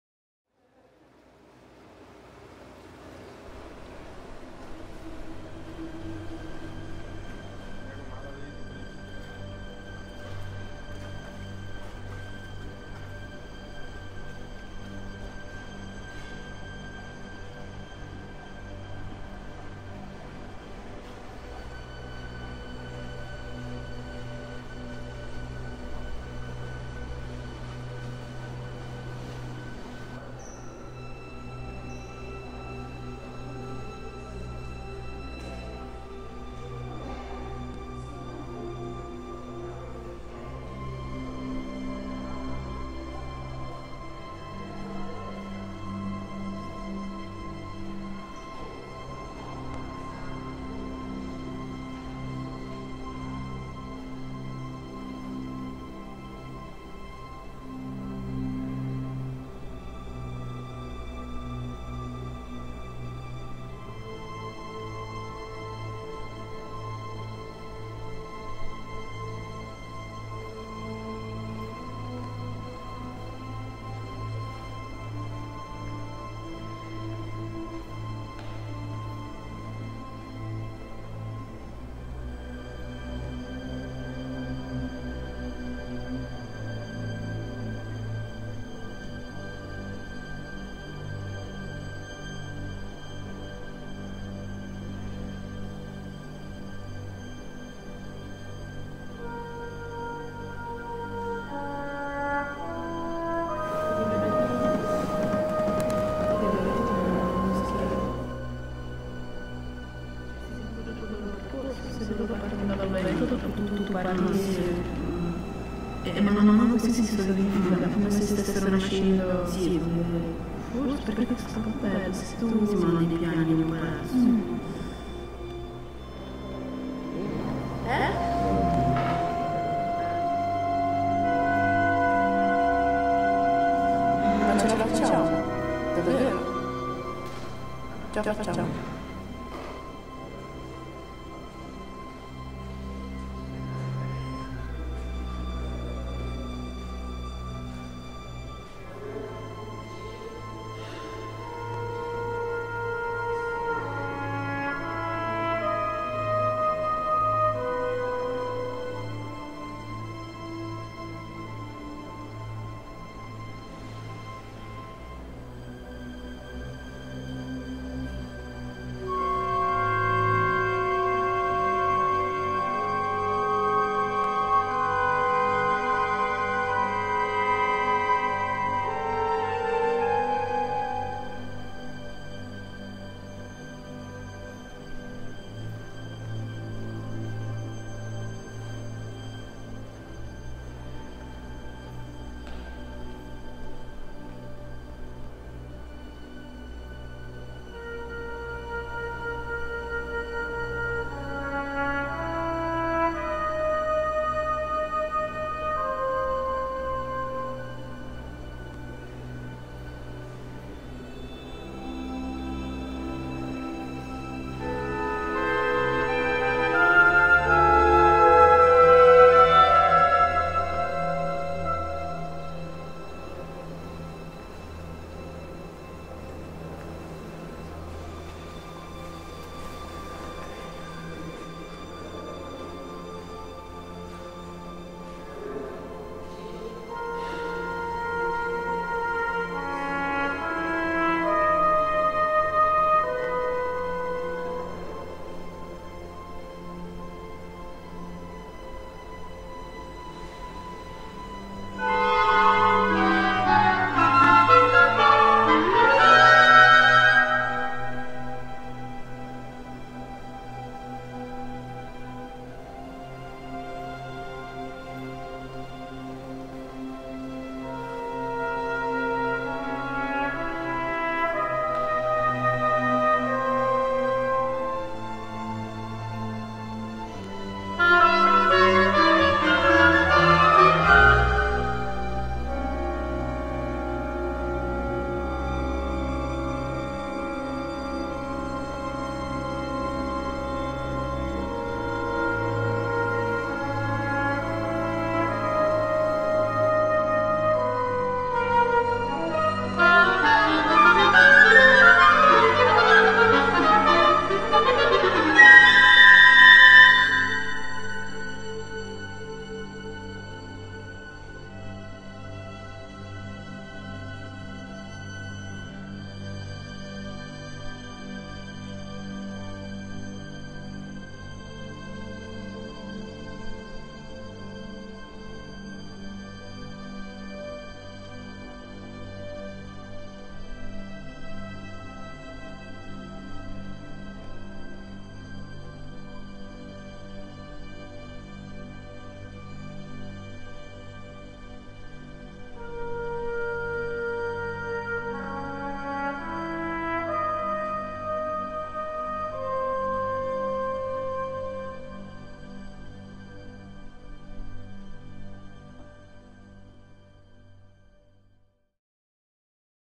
The perfect moment - Il momento perfetto
Video realizzato per l'esame finale del laboratorio video CLAVeS FdA IUAV, anno accademico 2009/2010, Prof: Massimo Magrì.
Riprese fatte in collaborazione con Musei Civici Veneziani a Ca' Pesaro di Venezia.
Cast: Marco Omiccioli, Anna Travaglia, Marzia Andreetta, Maryna Yakymhak, Ana Blagojevic.